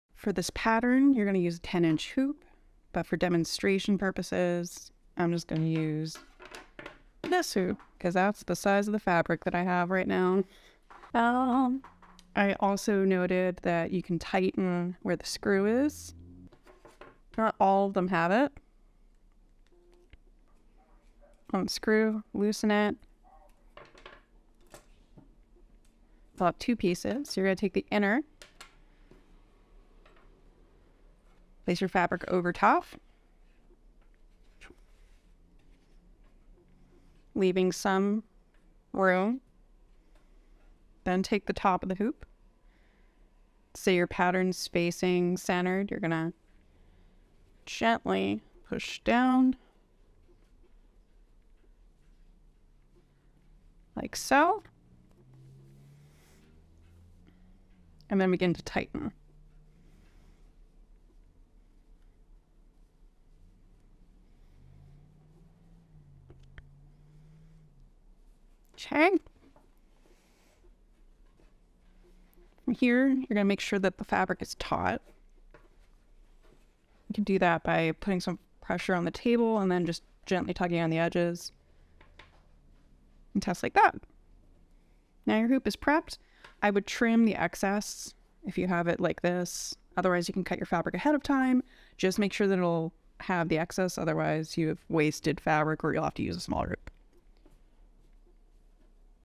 Hand Embroidery Hoop Prep
Setup an Embroidery Hoop for a successful project! A beginners guide to hand embroidery

Pattern coming April 2023! Consider following to support if you found this tutorial helpful.

Let me know if this video helped you!